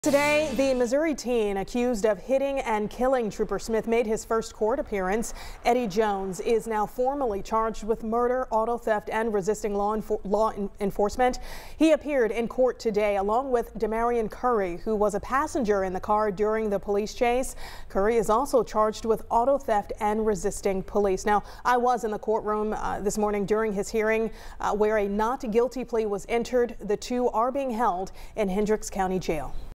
Teen accused of killing Trooper Smith makes first court appearance
Eddie Jones is now formally charged with murder, auto theft and resisting law enforcement.